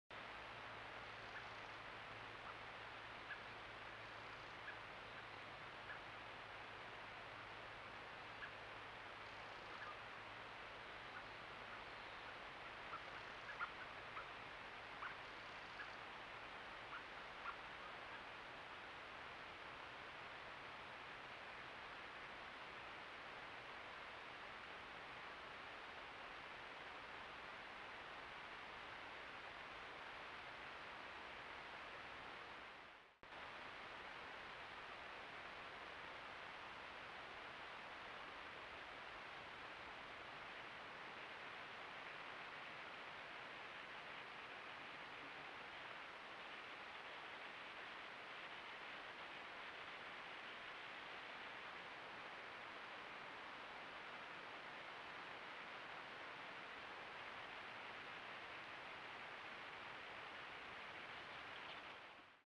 Pine Marten passing a trail camera site near Limerick City
Pine Marten passing a trail camera site near the Lower River Shannon, Co Limerick.